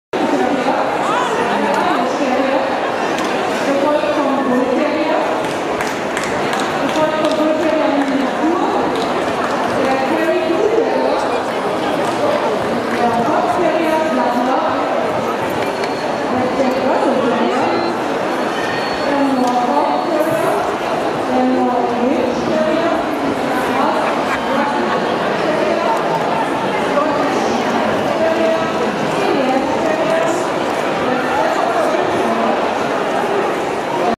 Iona High Speed "Morris" entering the ring for Best Junior terrier
Our young Jack Russell terrier Iona High Speed "Morris" made his debut at VDH Int Dog Show gaining Best Junior in breed and he also gained Reserve CAC and was runner up BOB Jack Russell terrier.